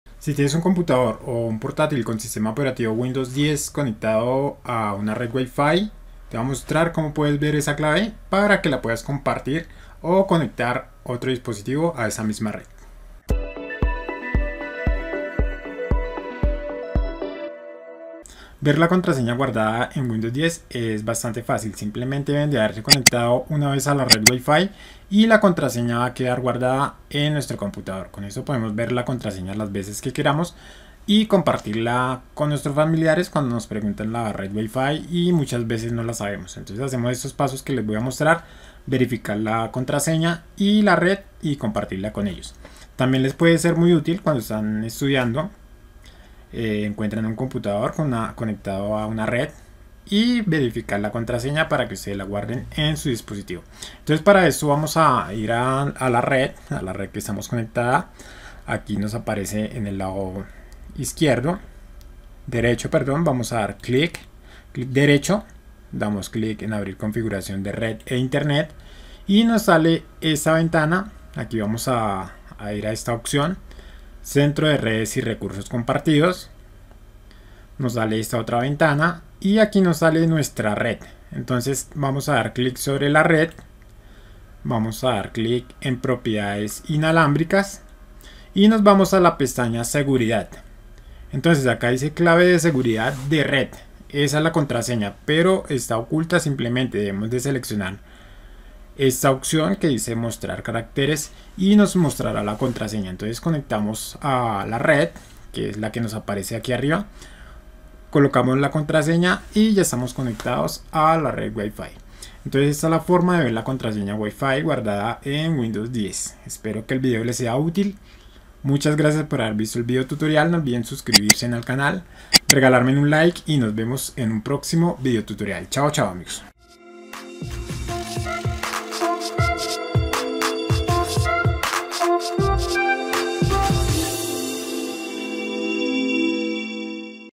📶 Cómo Ver La Contraseña WiFi Guardada En Windows 10 Propiedades de red inalámbrica
¿Olvidaste la contraseña de tu WiFi o necesitas compartirla con alguien más? En esta guía paso a paso, aprenderás cómo ver la contraseña de tu red WiFi en un sistema operativo Windows 10. No necesitas ser un experto en informática para seguir los sencillos pasos que te mostraremos en esta guía. Además, te mostraremos cómo recuperar la contraseña de redes WiFi a las que te hayas conectado anteriormente en tu computadora. ¡Sigue leyendo para recuperar la contraseña de tu red WiFi y mantener tus dispositivos conectados sin interrupciones!

👉 como ver la contraseña wifi guardada en mi pc en Windows 10

Capítulos

0:00 - Introducción
0:12 - Intro
0:12 - ¿Cómo se Almacena la Contraseña WiFi en Windows 10?
0:53 - Método para Visualizar la Contraseña WiFi en Windows 10
1:55 - Agradecimientos y Despedida